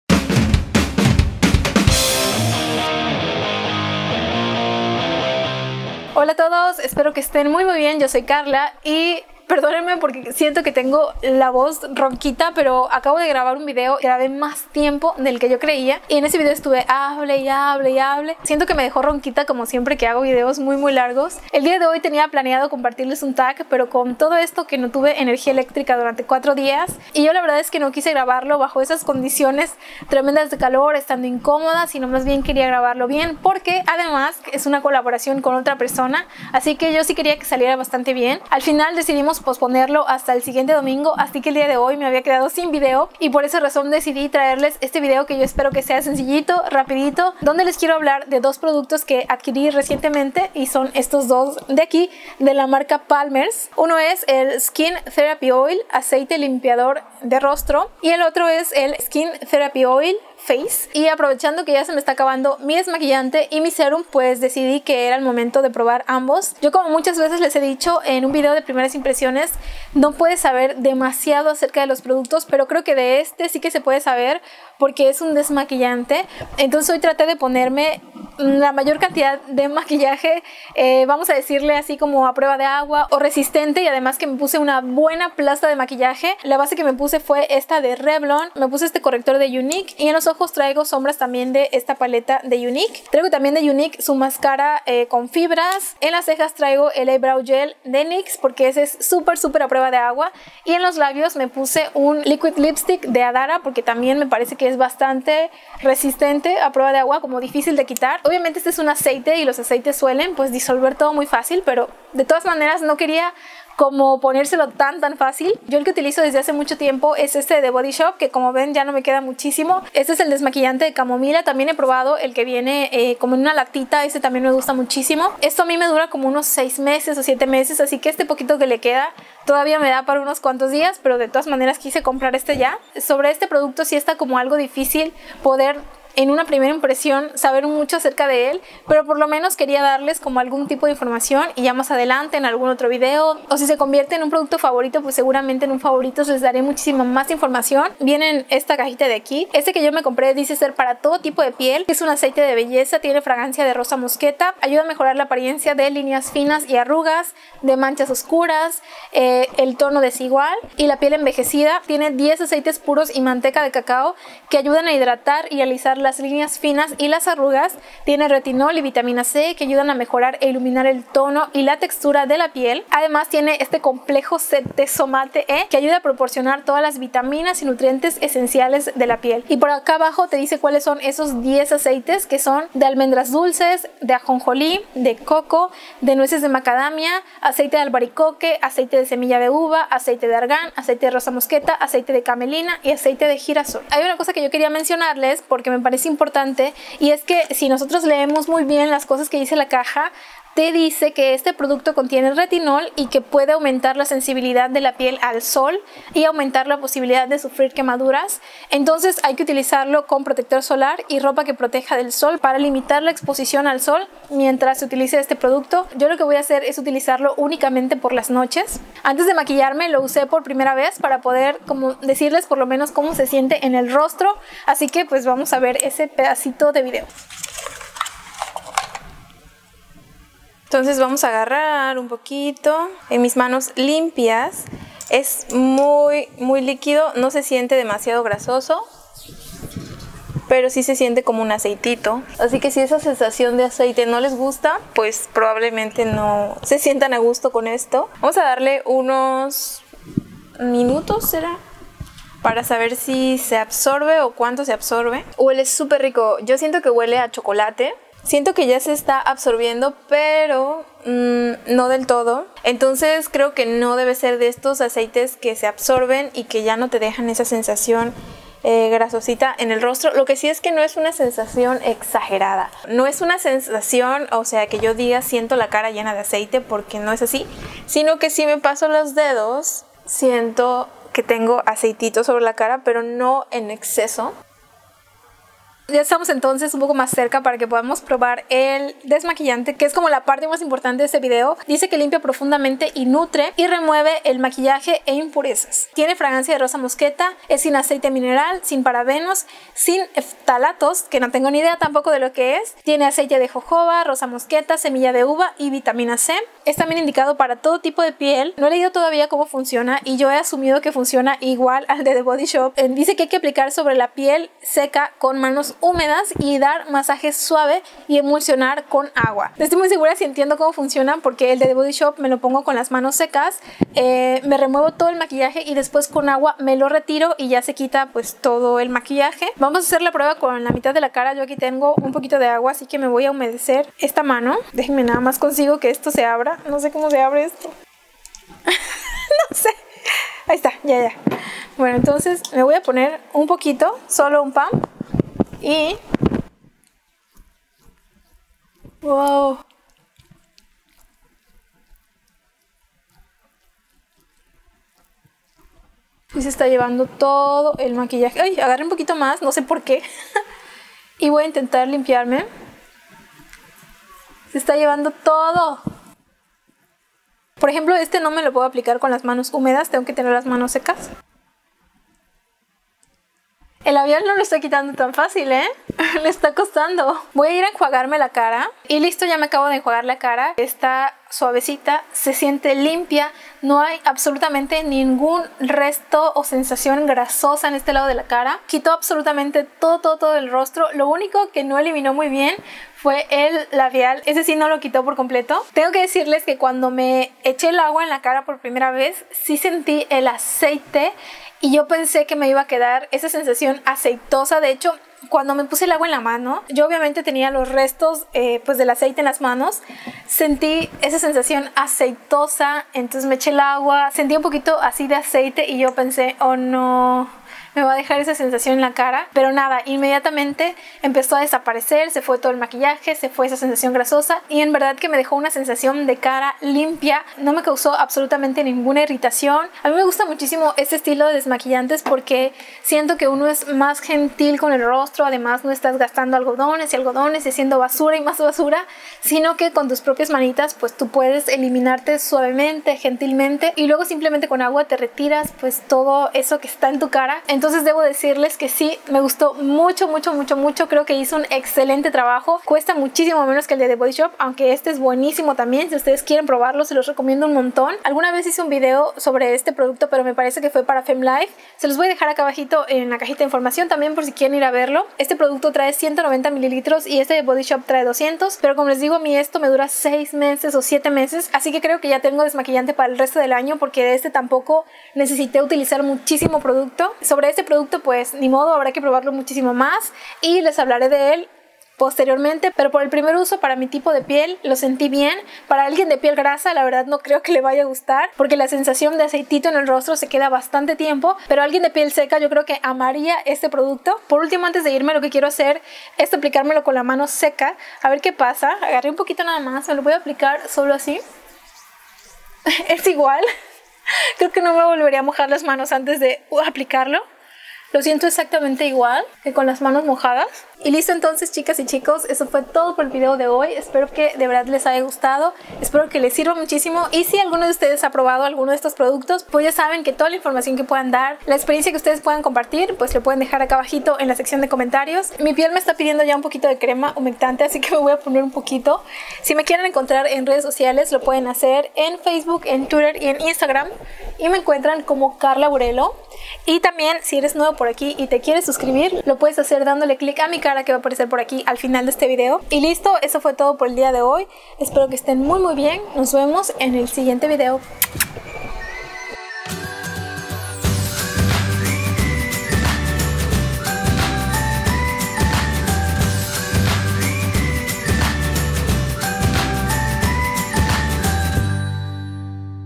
Aceite Limpiador (desmaquillante) y Aceite Multipropósito Skin Therapy Oil de Palmer's + Demo
Holaa!! En el video de hoy les dejo mis primeras impresiones del Skin Therapy Oil Aceite Limpiador (desmaquillante) y Aceite Multipropósito de Palmer's + Demo de cada uno de los productos...

Kar :)

- ¿Donde los compré?: Mega Comercial Mexicana (también los vi en Walmart)
- Precio: Limpiador 117 MXN / Aceite Facial 125 MXN

- ¿Qué labial traigo?: Liquid Lipstick Adara / Tono 21
- ¿Que traigo en las uñas?: Esmalte Olé! / Tono GRS212